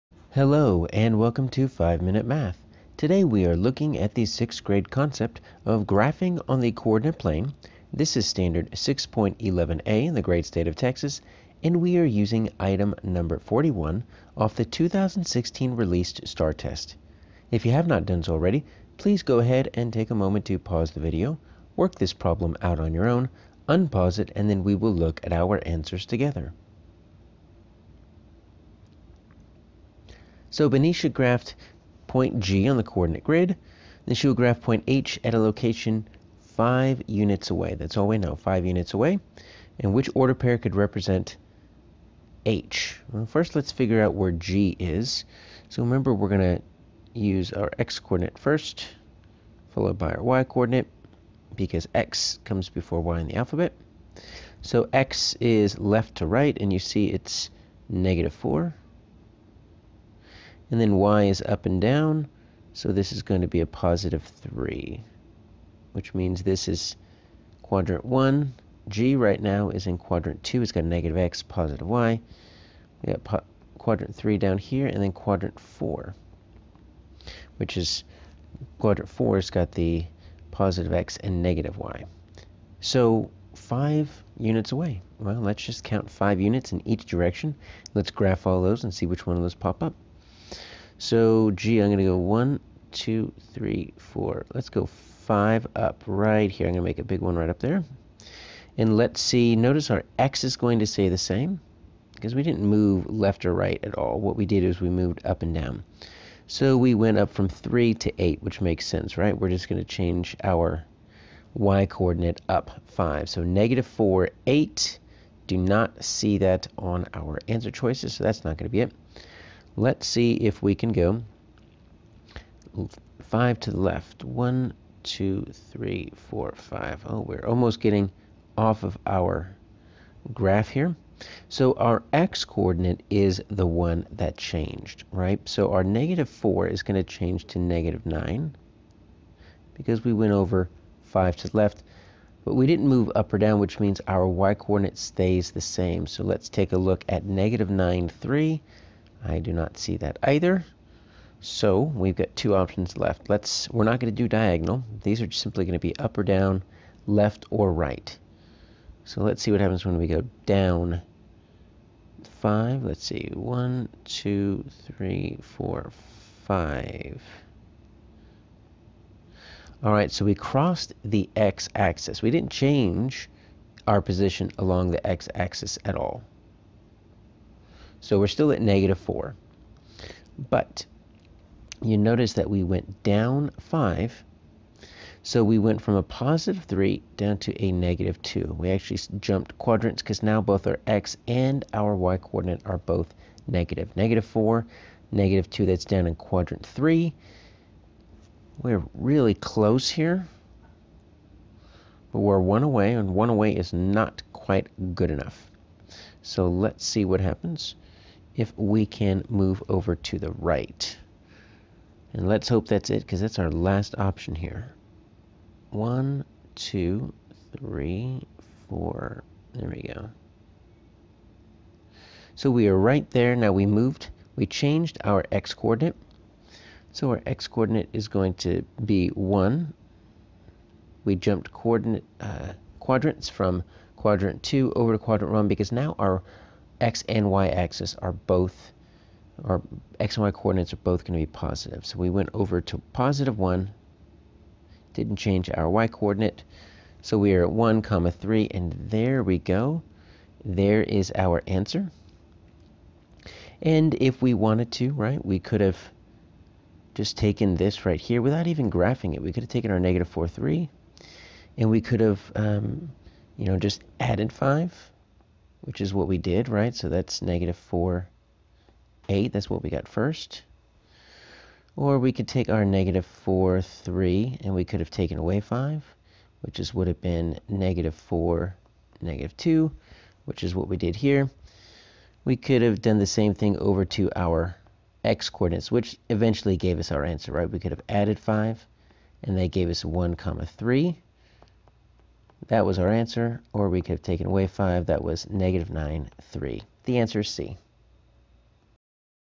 6th Grade STAAR Practice Graphing on the Coordinate Plane (6.11A - #2)
Practice graphing points in all four quadrants using ordered pairs of rational numbers with a test item from the 2016 released STAAR test (#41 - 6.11A).